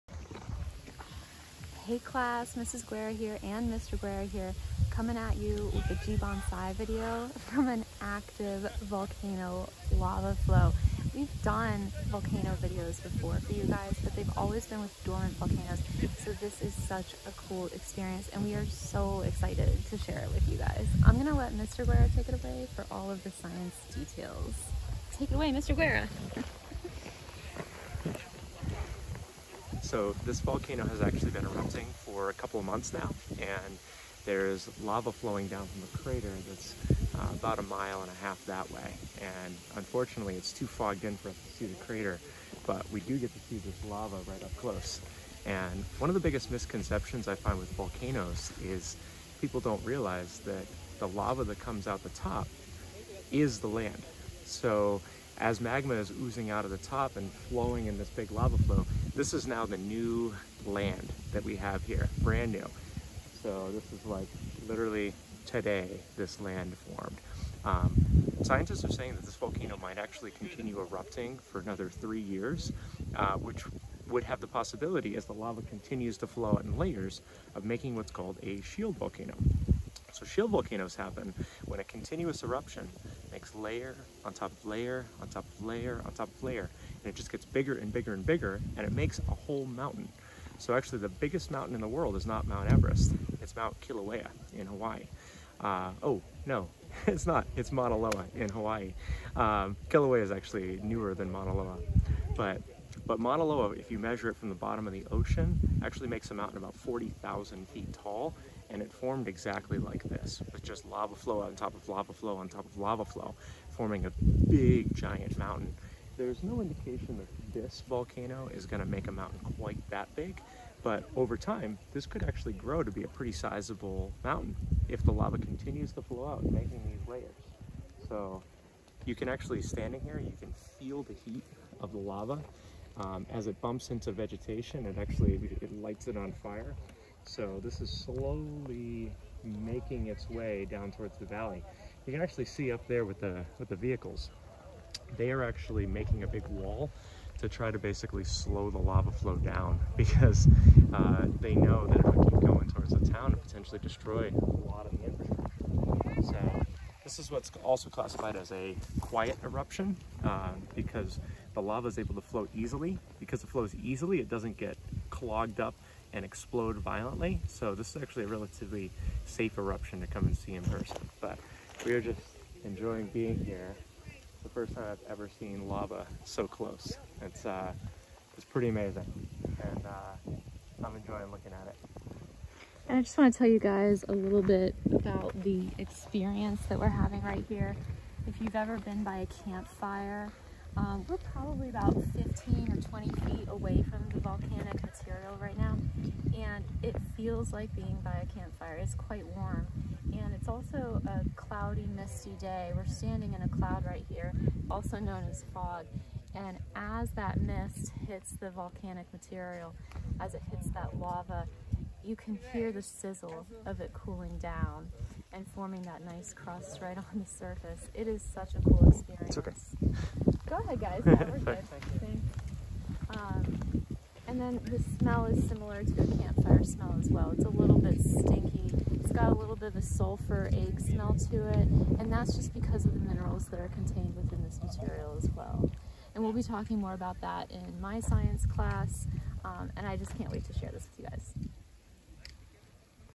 How Volcanoes Create New Land - Live from Iceland
Mr. and Mrs. Guerra show a lava flow in Iceland and describe how eruptions create new land.